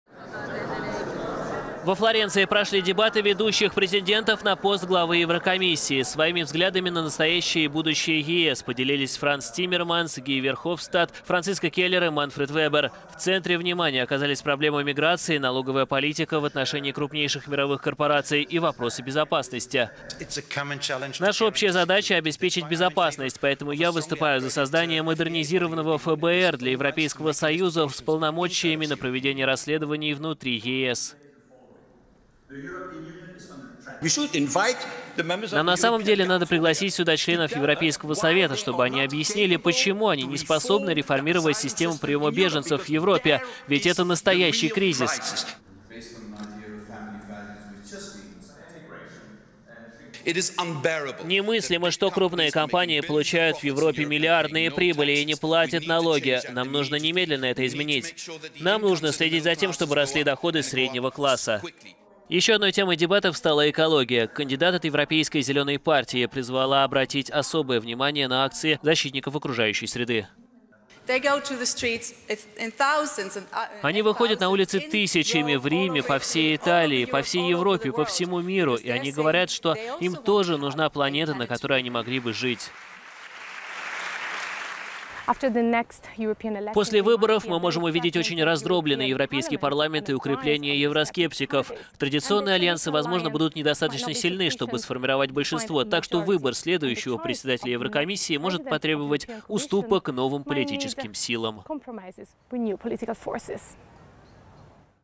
Европейское ФБР, беженцы и налоги
Во Флоренции прошли дебаты основных претендентов на пост главы Еврокомиссии…